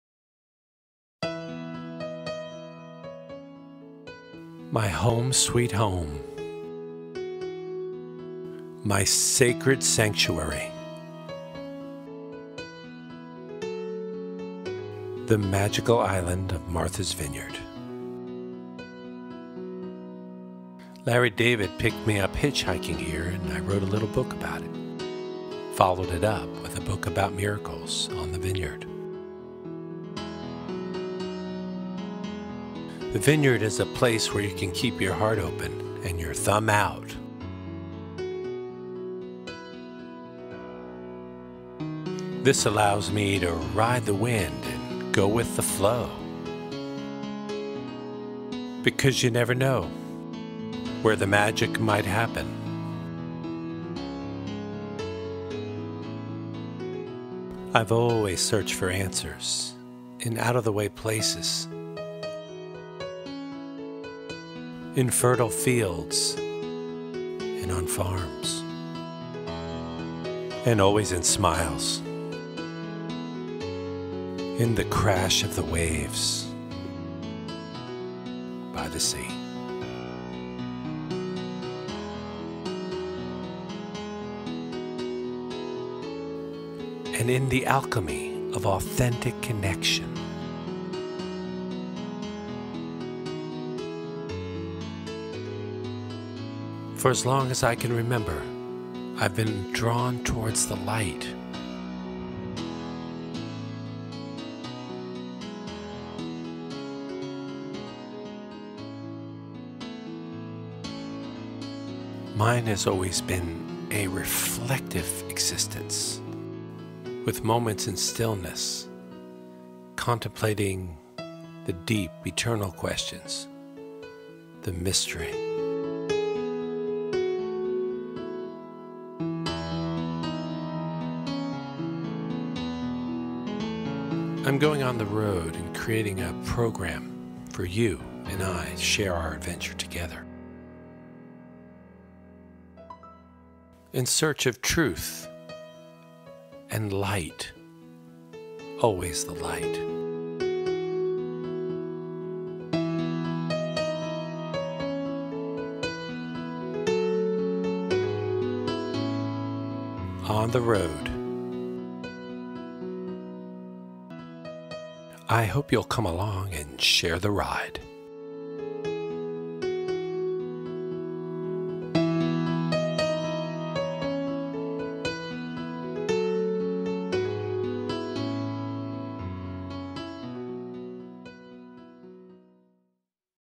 On The Road with Paul Samuel Dolman
Enjoy my love letter to the Vineyard which also doubles as the promotional video for my upcoming video series... On the Road... Come with on a tour of the Vineyard and behold the beauty of this sacred place!